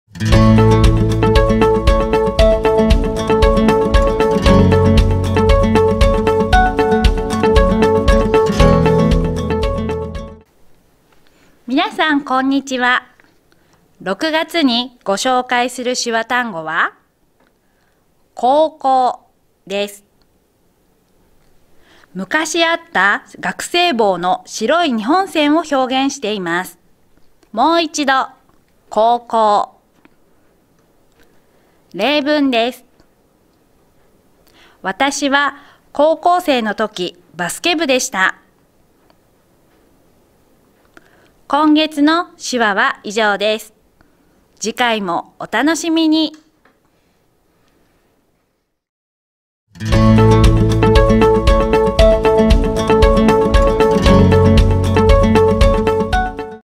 今月の手話「高校」 令和６年６月１日号
広報しぶかわ　2024年６月１日号　今月の手話Vol.86
手話で「高校」という表現を学びます。

■前回の手話動画「中学校」